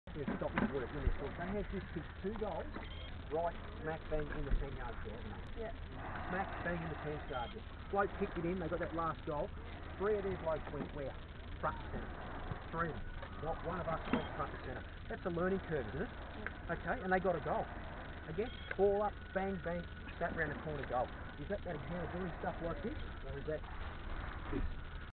Under 14 Huddle Collendina Cobras 20/07/2013 V Lara
Under 14 three quarter time Huddle Collendina Cobras 20/07/2013 V Lara